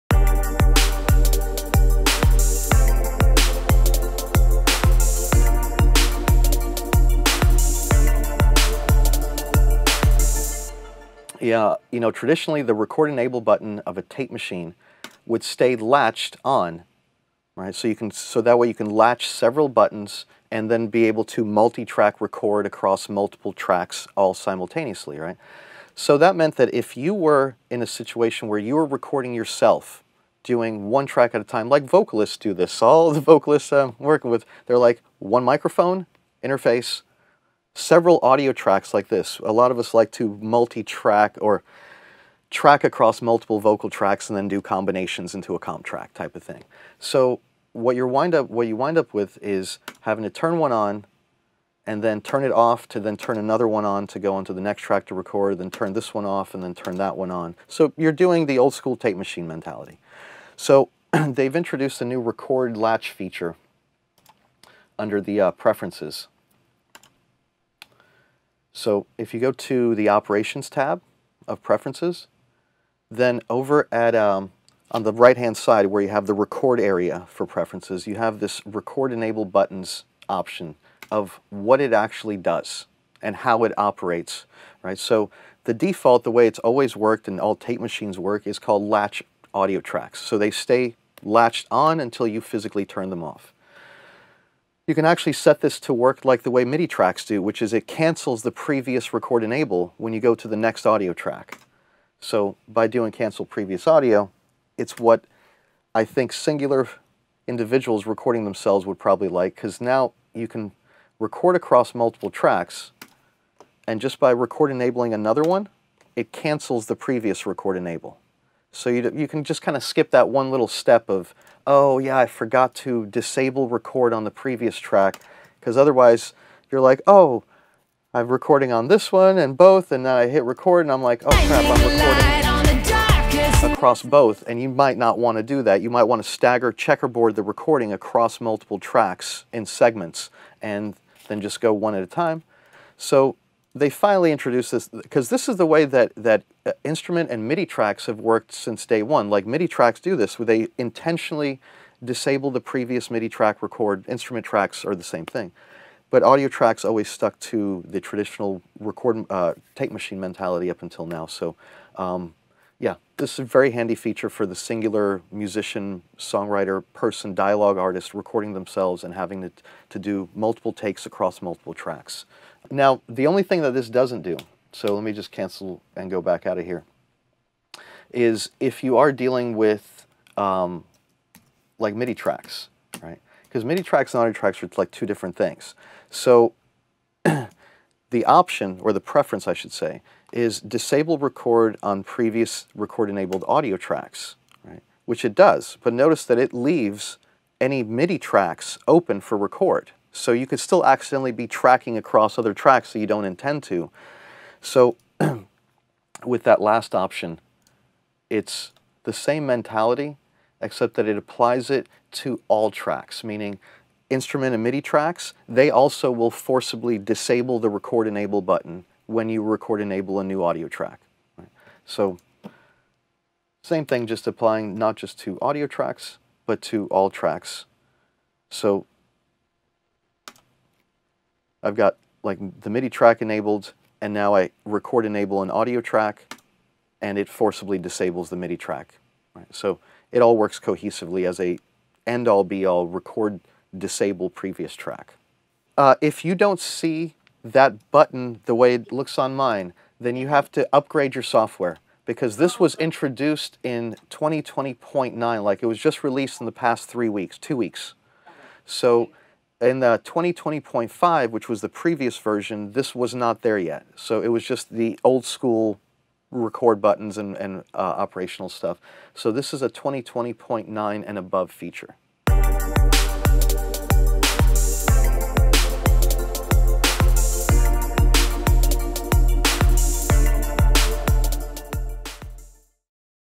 Record Options In Pro Tools 2020
The release of Pro Tools 2020.9 has brought some handy new features that let you set how the Record Enable button on Audio Tracks operates. It can now un-latch the previously record enabled audio tracks, allowing a single user an easier time of recording across multiple tracks in one session.